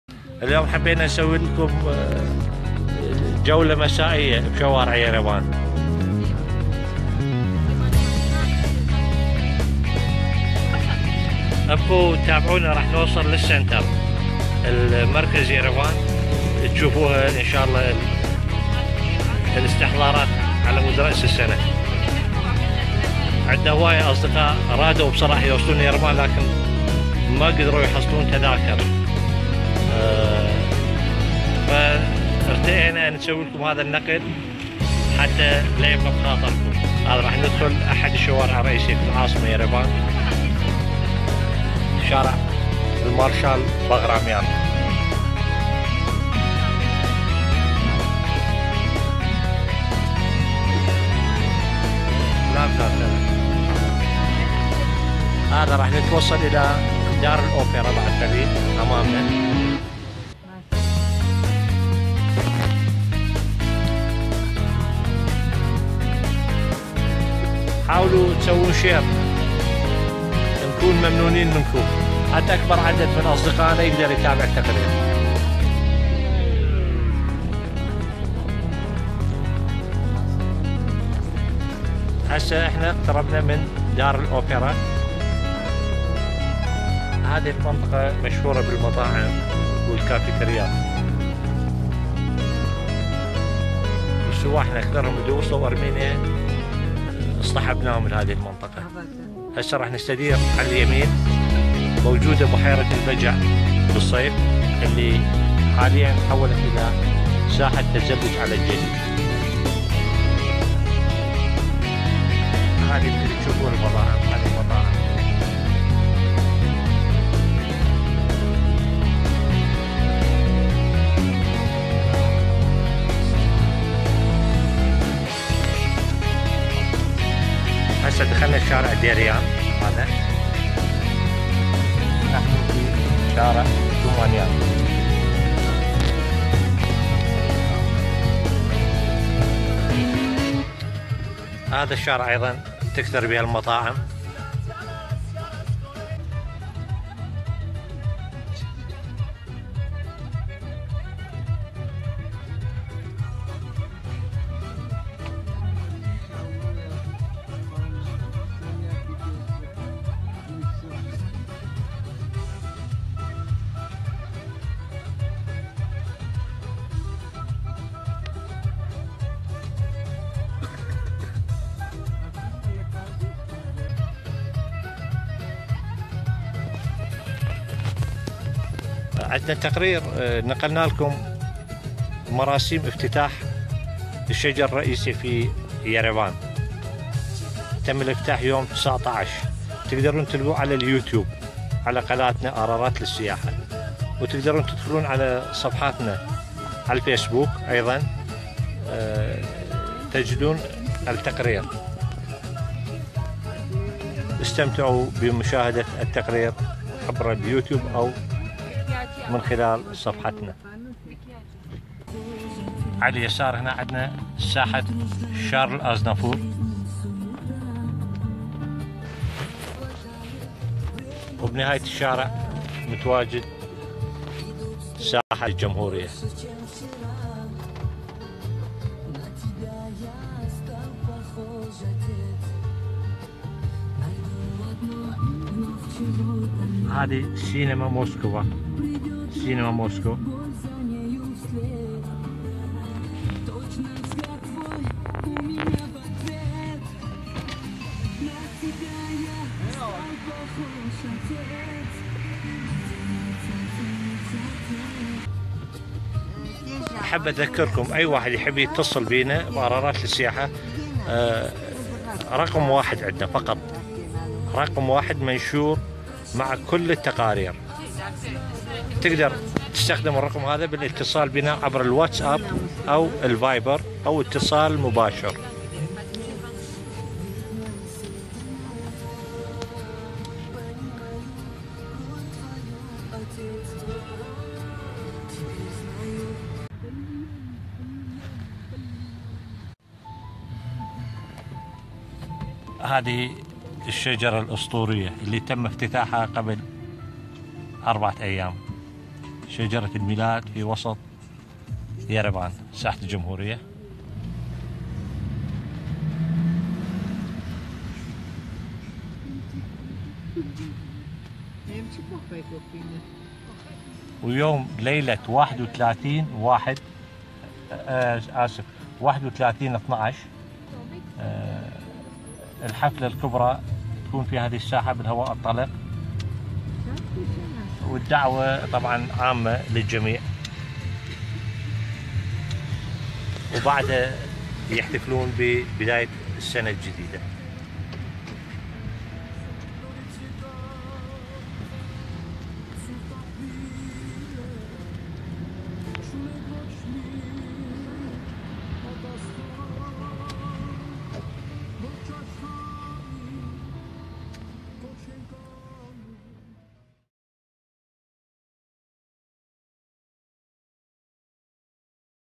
ارمينيا / جولة في شوارع يريفان
من خلال هذا الفيديو ننقل لكم مشاهد مسائية في مدينة يريفان حيث تجولنا بالسيارة في  شوارع
مختلفة في يريفان
شارع باغراميان وشارع ديريان
مرورا ب ساحة الجمهورية وسط يريفان
اعداد وتصوير : ارارات للسياحة